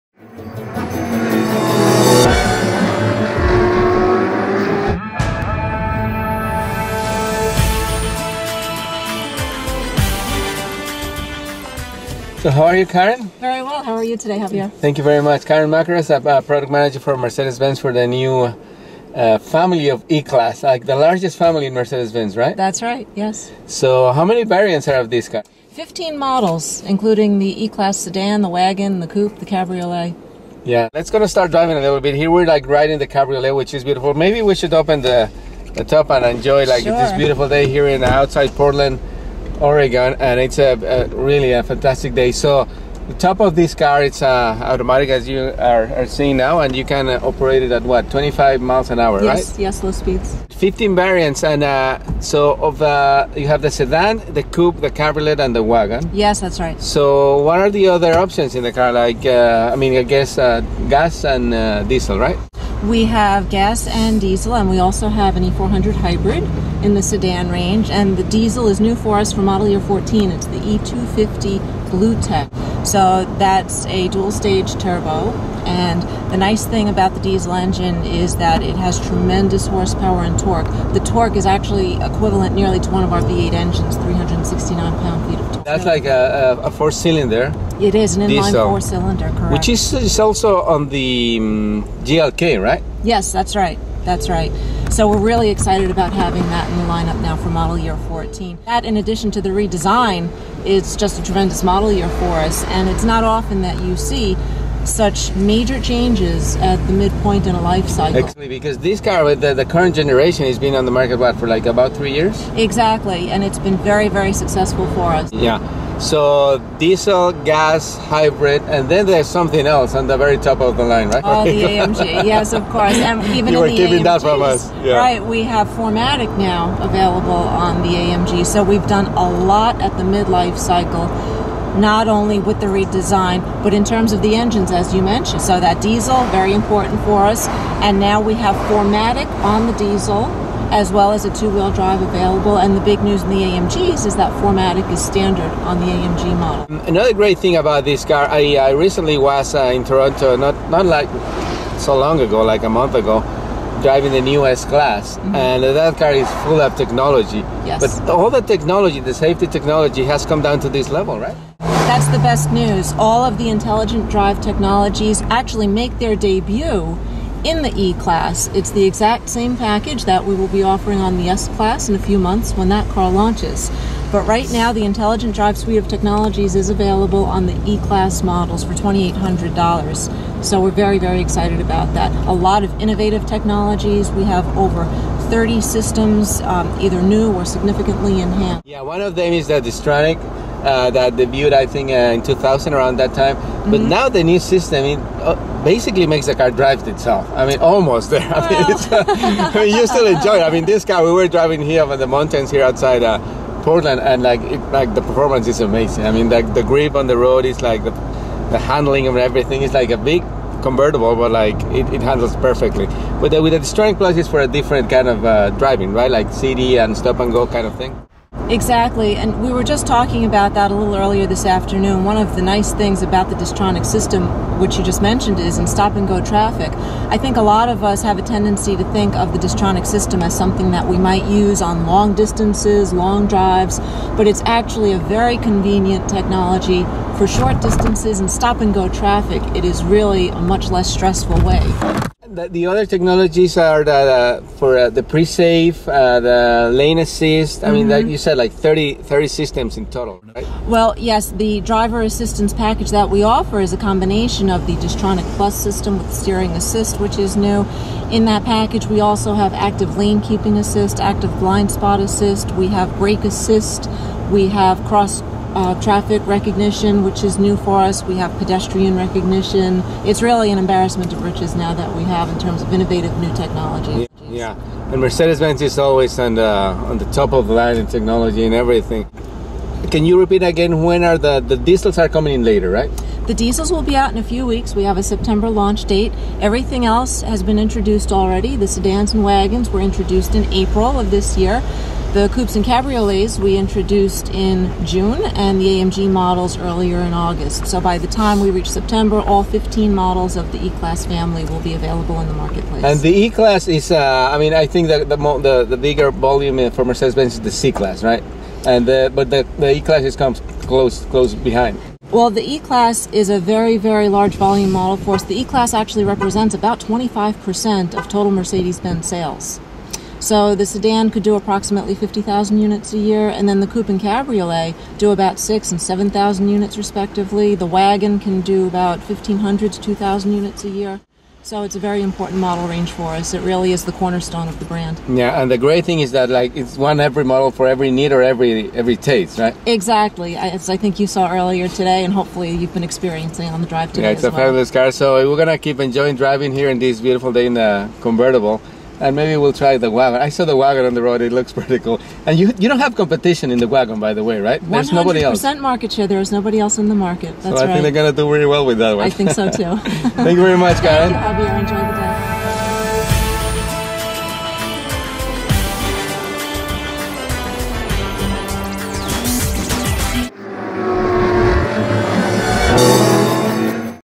2014 Mercedes-Benz E Class Full line
An interview with Karen Makris, Mercedes-Bnez Assistant Product Manager for the E- Class Family, which has been extensively updated for 2014.

Many new or optimized assistance systems from the future S-Class celebrate their world premiere in all E-Class models.

They blend comfort and safety. Mercedes-Benz refers to this as "Intelligent Drive." This includes systems capable of preventing accidents with crossing vehicles or pedestrians for the very first time.

Active Lane Keeping Assist can also prevent collisions with oncoming traffic. With these innovations Mercedes-Benz underscores its pioneering role in vehicle safety.